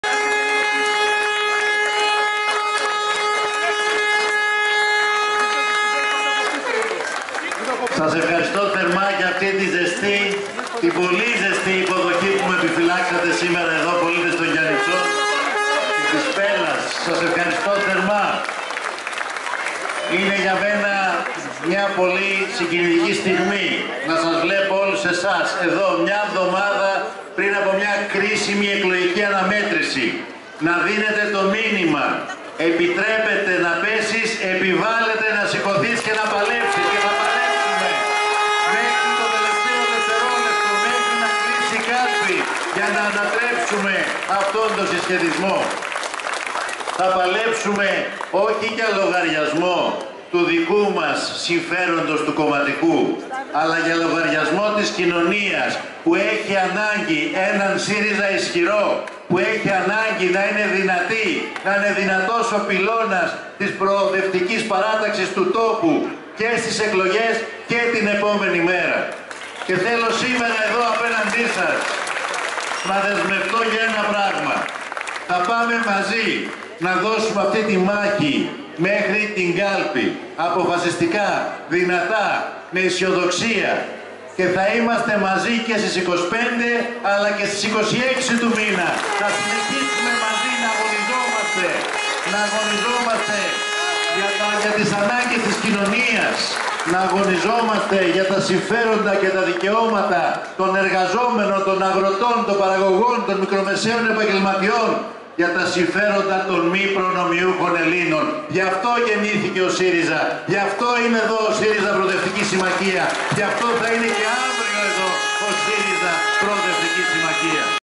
Αλέξης Τσίπρας: Επιτρέπεται να πέσεις, επιβάλλεται να σηκωθείς και να παλέψεις | 17/6/2023 (2)
Βίντεο:
Αντώνης Παπαδόπουλος
(Συμβαίνει Τώρα)
Σάββατο 17/6/2023

Επίσκεψη Αλέξη Τσίπρα στα Γιαννιτσά
ΣΥΡΙΖΑ ΠΡΟΟΔΕΥΤΙΚΗ ΣΥΜΜΑΧΙΑ

Χαιρετισμός του κόσμου και ομιλία
στον πεζόδρομο, Πλατεία ΕΠΟΝ.